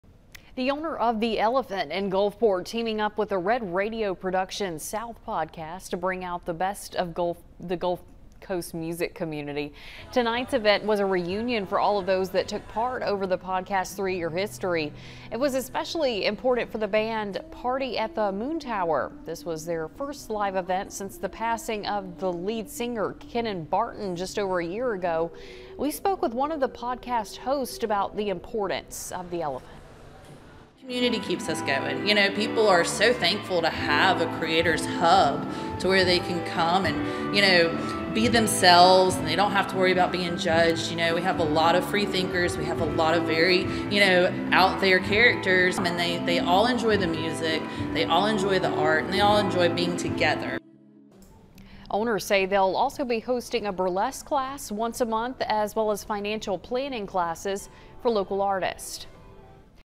The Elephant bringing out the best in Gulf Coast music
The owner of the Elephant in Gulfport teamed up with the Red Radio Production South Podcast to bring out the best of the Gulf Coast music community.